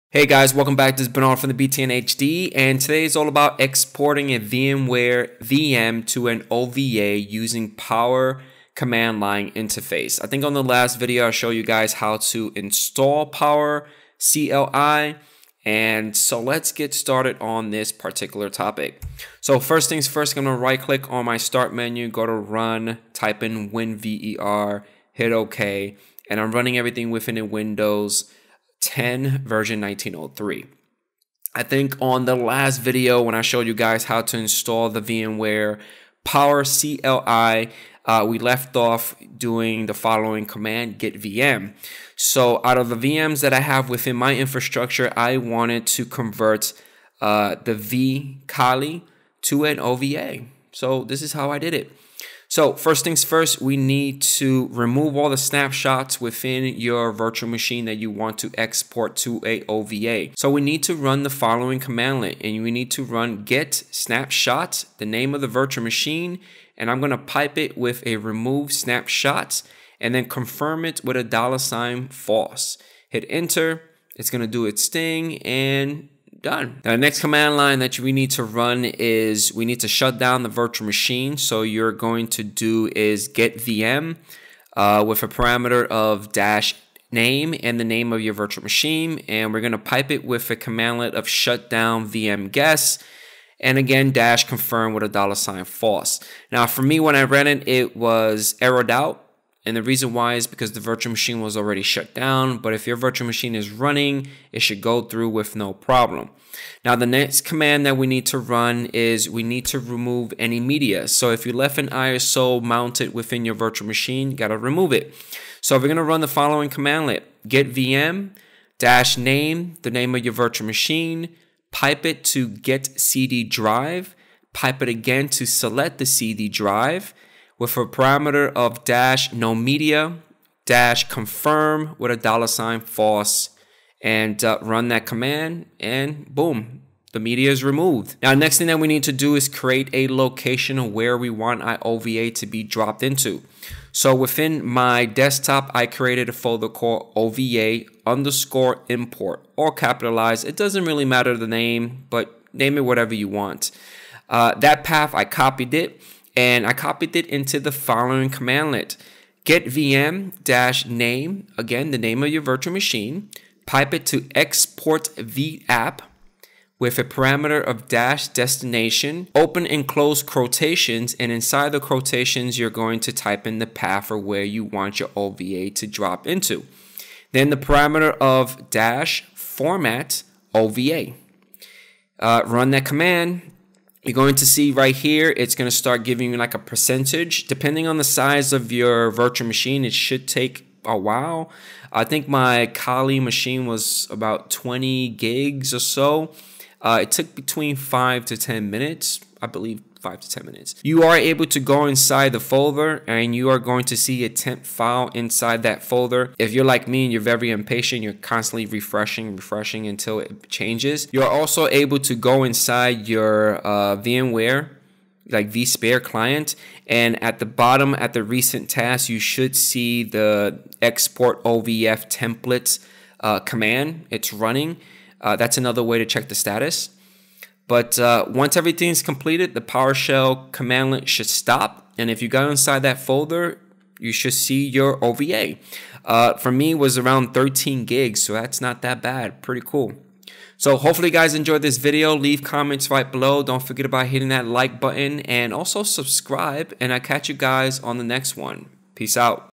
Export a VMware VM to OVA with PowerCLI!
We're doing more VMware PowerCLI stuff and today we will go over how to export a VMware VM to OVA with PowerCLI.